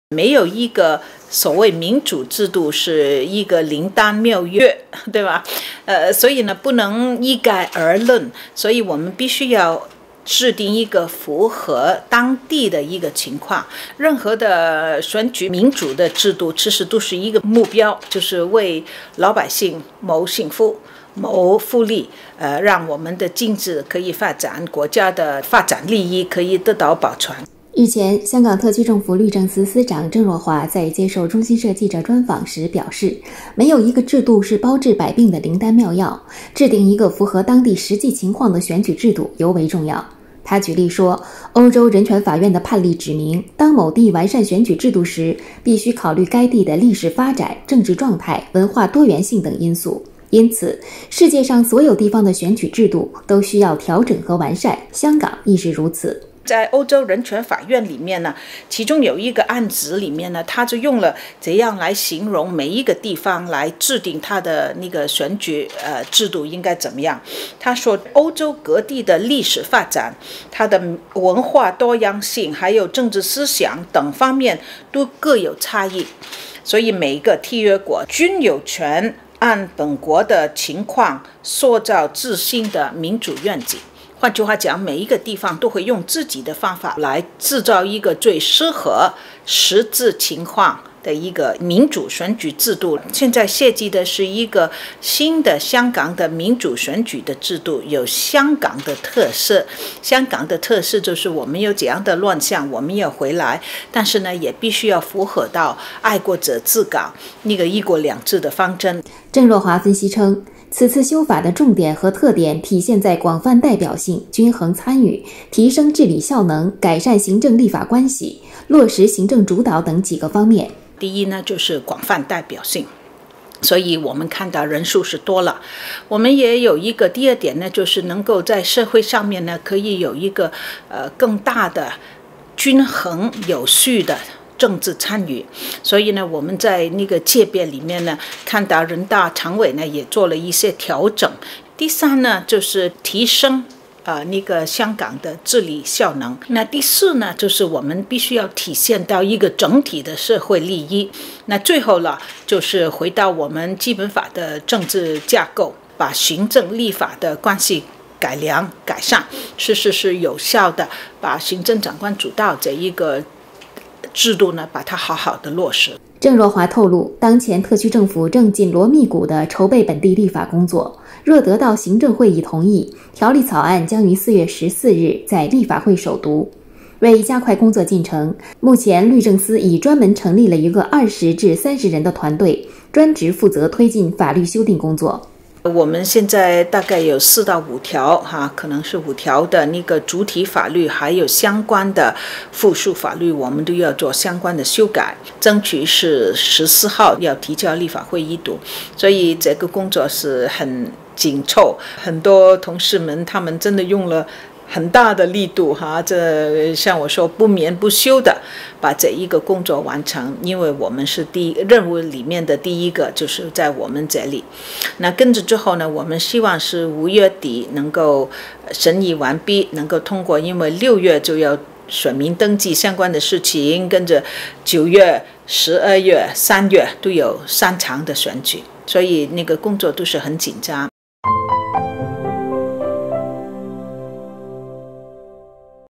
郑若骅：制订符合香港特色的民主选举制度至关重要
日前，香港特区政府律政司司长郑若骅在接受中新社记者专访时表示，没有一个制度是包治百病的灵丹妙药，制订一个符合当地实际情况的选举制度尤为重要。她举例说，欧洲人权法院的判例指明，当某地完善选举制度时，必须考虑该地的历史发展、政治状态、文化多元性等因素。因此，世界上所有地方的选举制度都需要调整和完善，香港亦是如此。